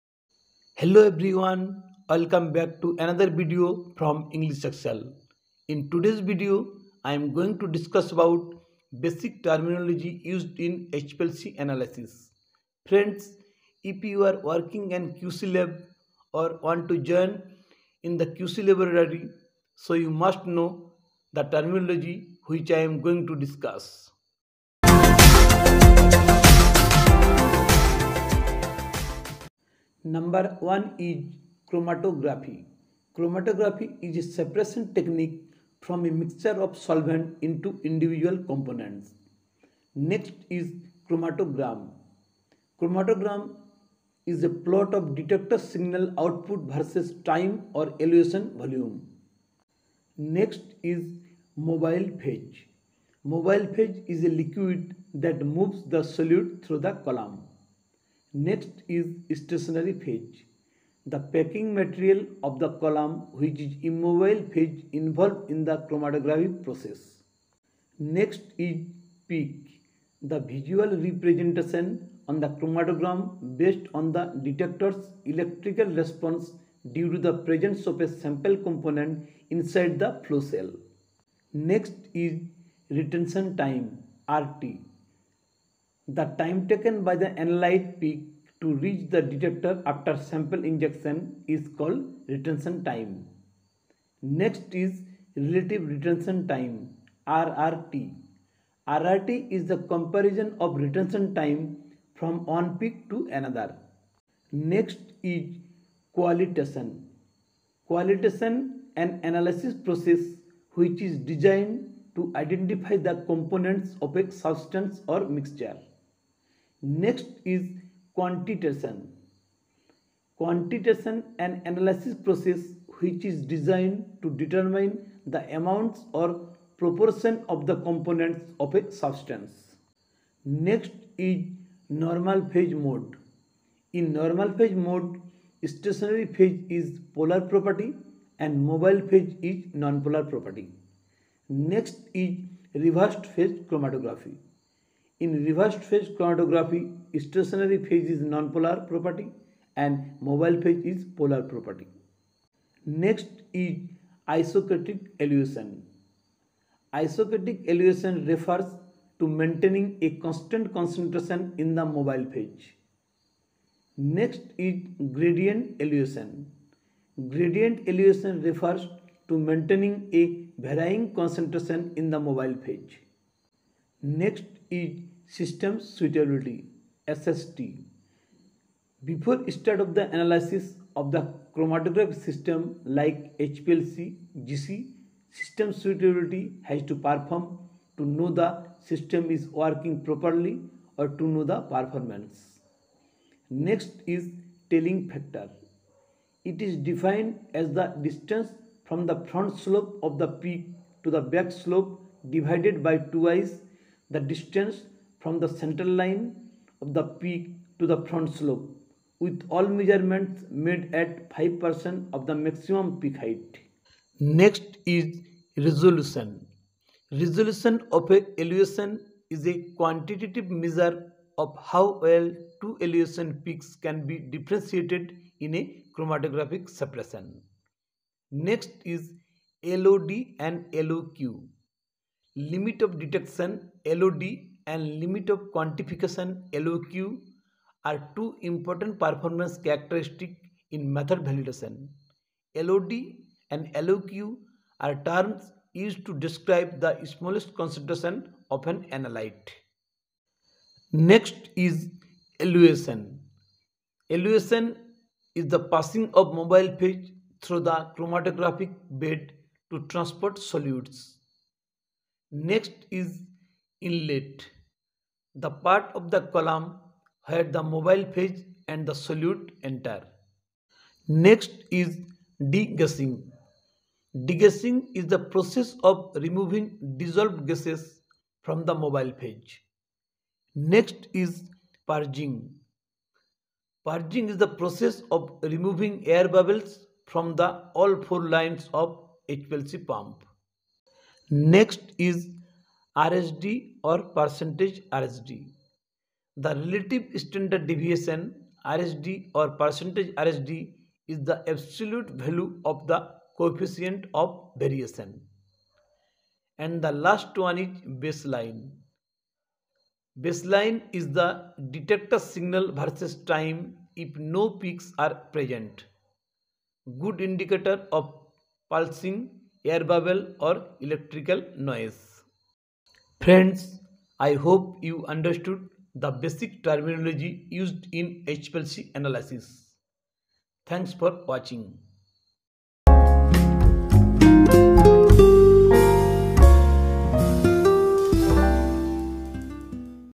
Basic terminology used in HPLC analysis | HPLC chromatography | Normal phase and reversed phase HPLC
Hello friends this video contains the basic terminology used in HPLC analyss. If you are working as a QC person and want to join in the QC depatment in Pharma industry than you must know the meaning and definition of various terms which are used in HPLC analysis.

Retentiontime(RT)
Relativeretentiontime(RRT)